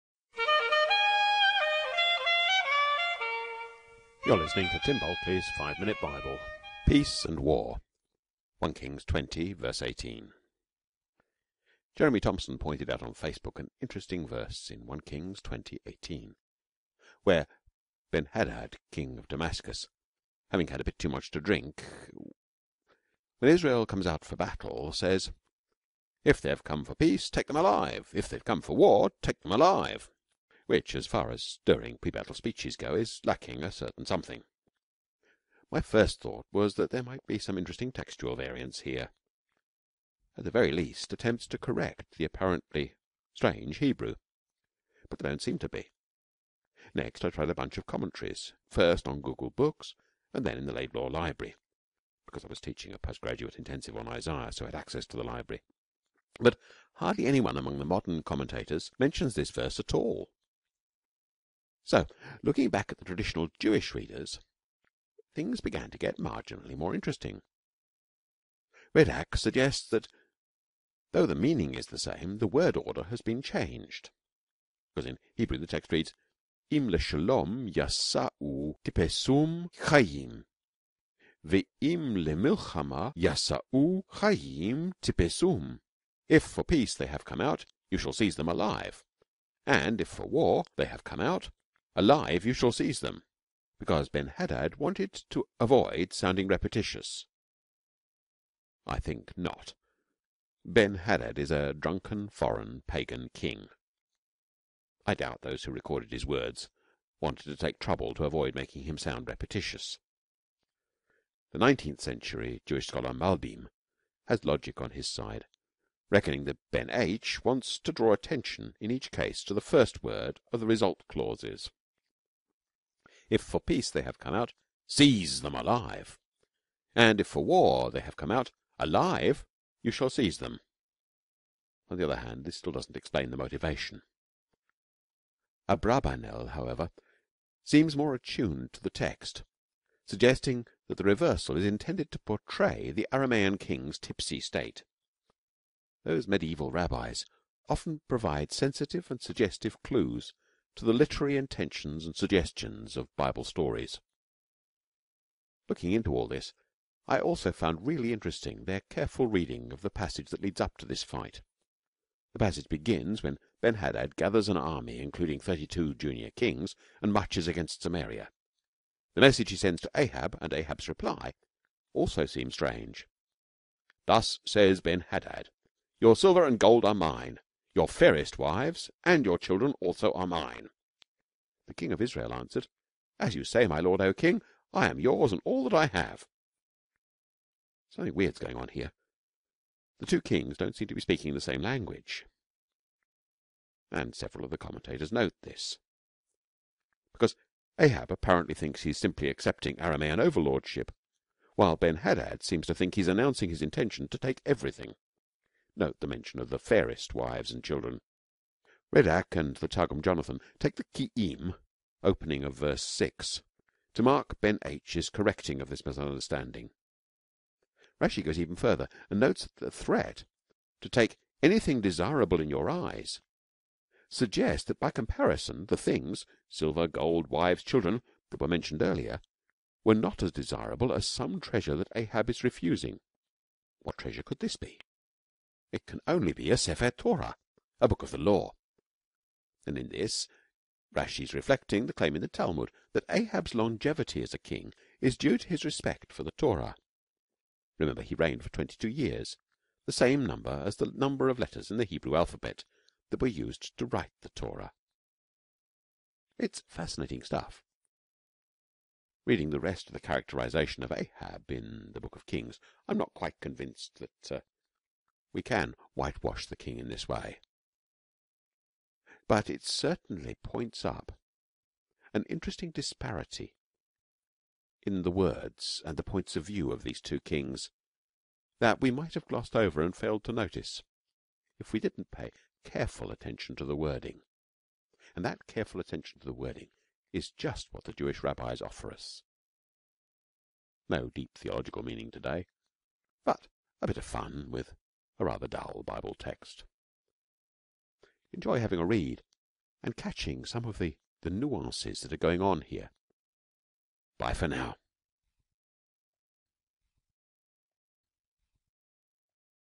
Peace and War: 1 Kings 20:18
A drunken, pagan foreign king and some smart rabbinic interpreters.